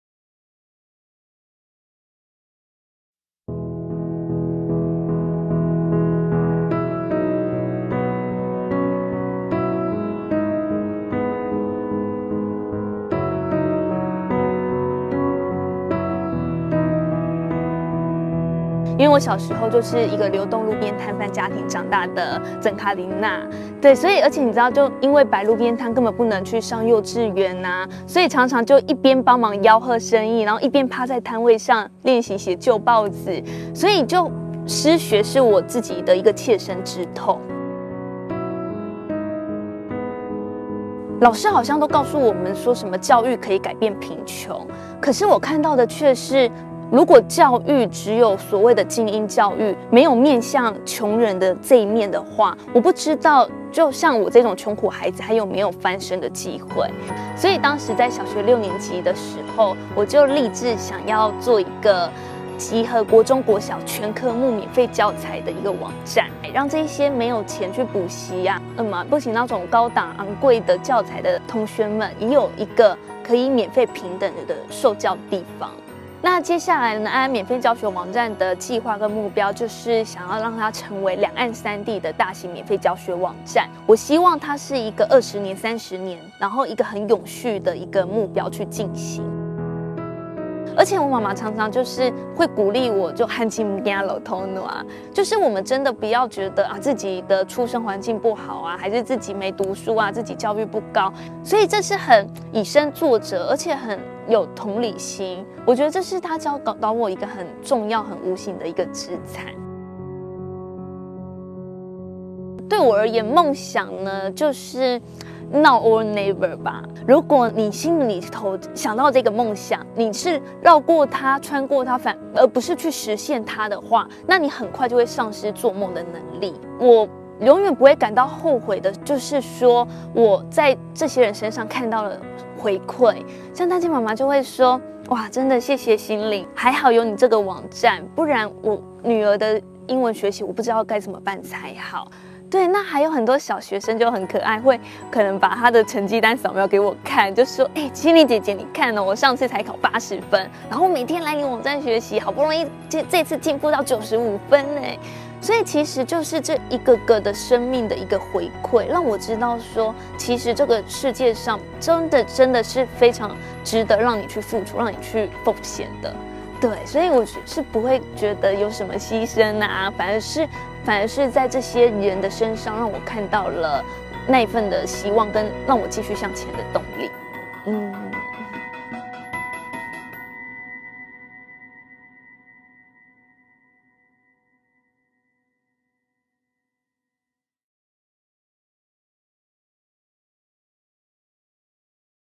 Keep Walking Fund - 沈芯菱(Shen Hsin-Ling)
成立兩岸三地大型的免費教學網站，讓百萬學子能透過網路資源獲得完善學習，改善城鄉差距所帶來的知識斷層。
Set up a free tutoring website for elementary and junior high school students throughout the Greater China region to address the education disparity between urban regions and rural villages.